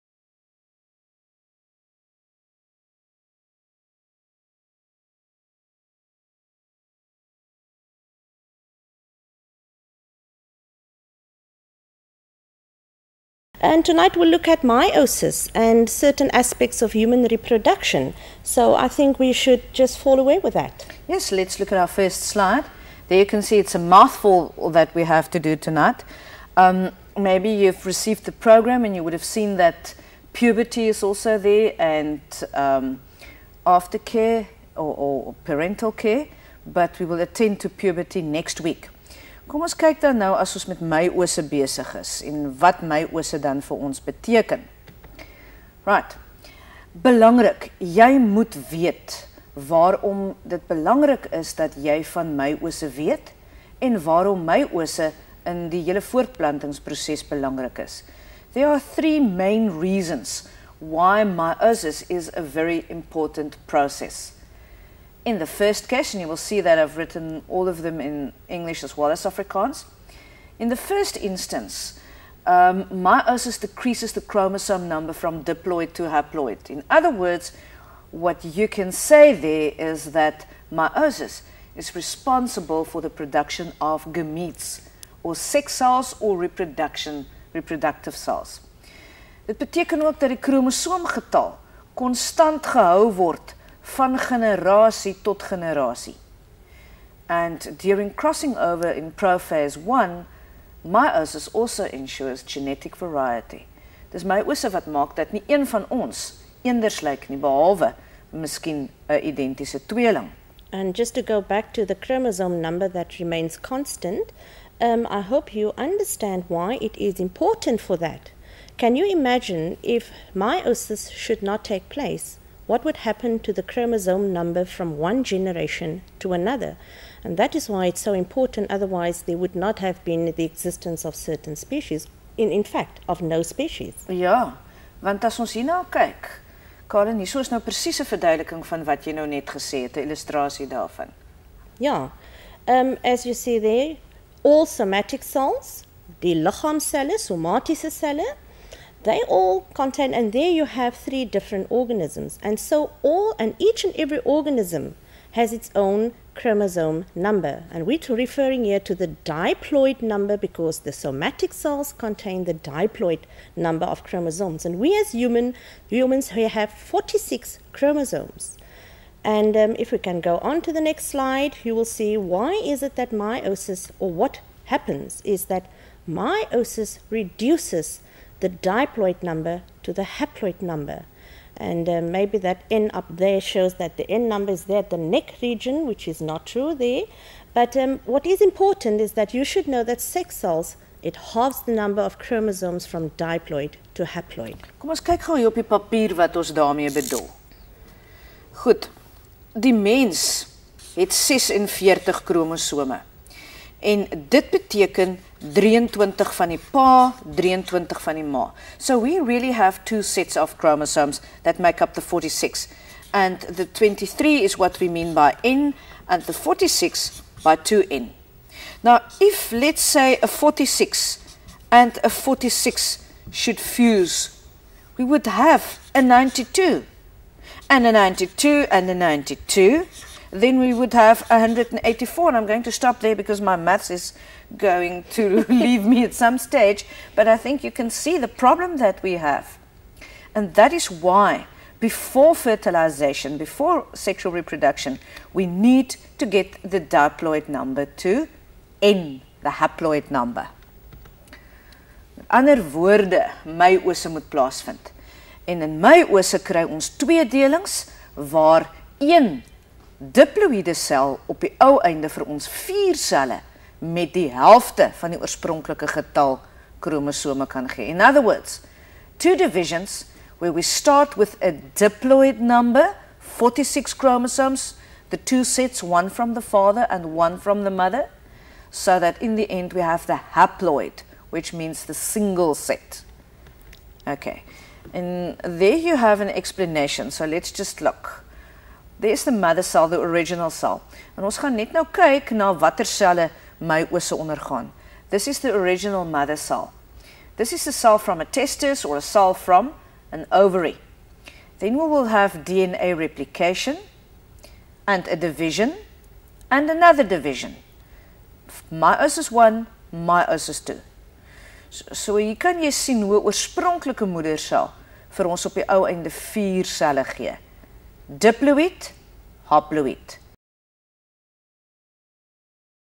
Matric Revision: Life Sciences: Meiosis (1/19): importance
Lesson conducted in English and Afrikaans: importance (diploid to haploid, chromosome number kept constant, ensures genetic variety).